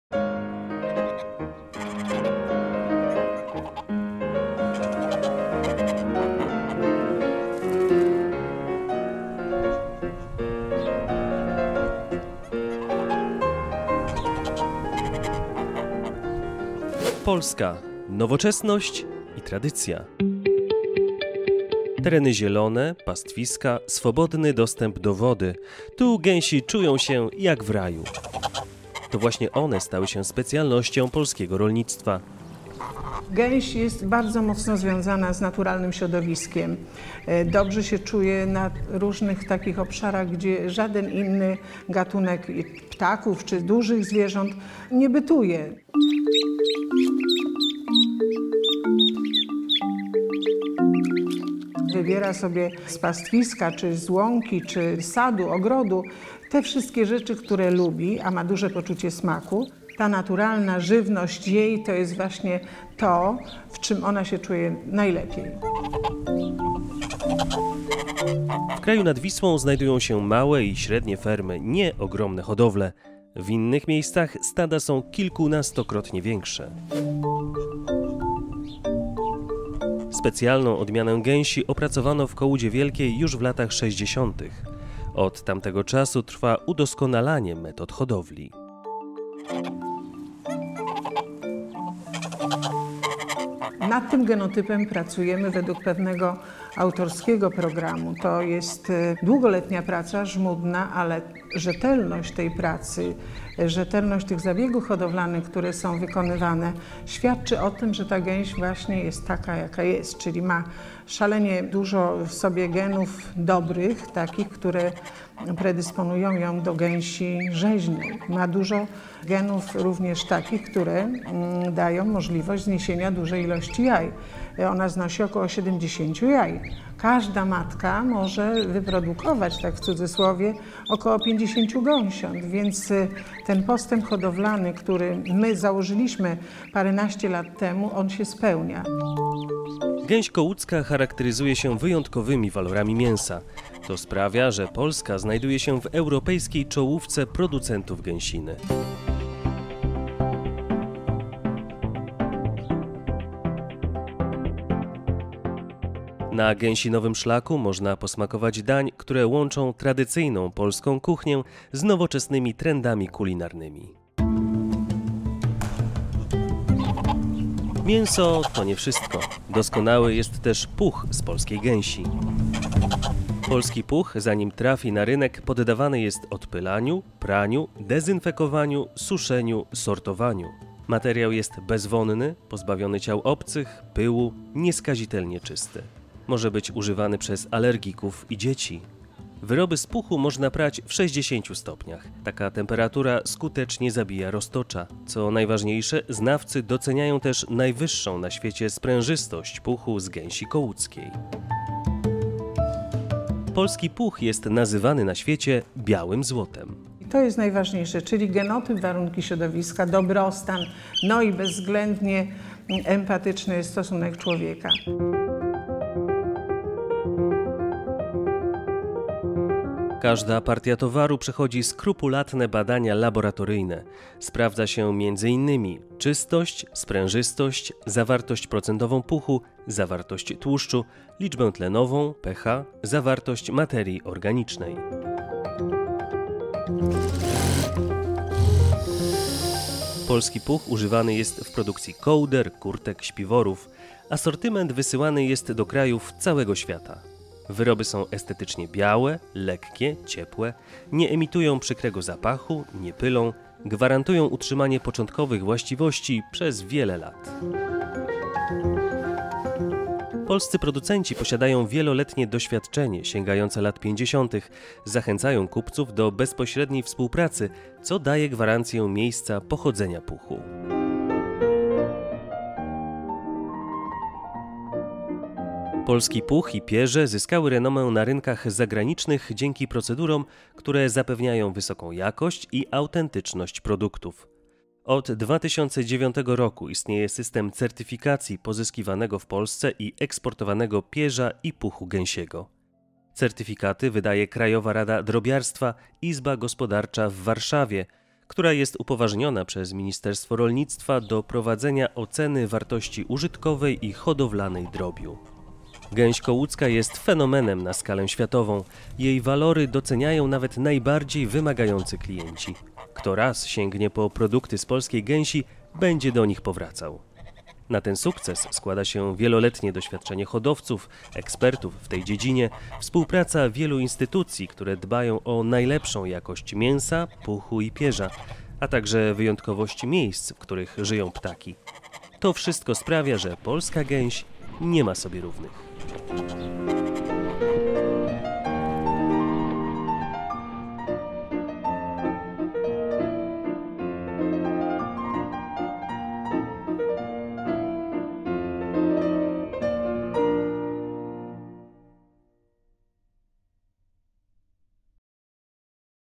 Skrzydła sukcesu - polska gęsina
Polska znajduje się w europejskiej czołówce producentów gęsiny. Jej najsłynniejszą odmianą jest gęś kołudzka, wyhodowana w latach sześćdziesiątych w Kołudzie Wielkiej. Mięso gęsi charakteryzuje się wyjątkowymi walorami. Białko, witaminy, minerały i tłuszcze w nim zawarte są nieocenione w walce z chorobami cywilizacyjnymi. Na rynkach zagranicznych renomę i uznanie zyskały również puch i pierze z polskiej gęsi. Od 2009 roku istnieje system certyfikacji pozyskiwanego w Polsce i eksportowanego pierza i puchu gęsiego, potwierdzający wysoką jakość i autentyczność tego produktu.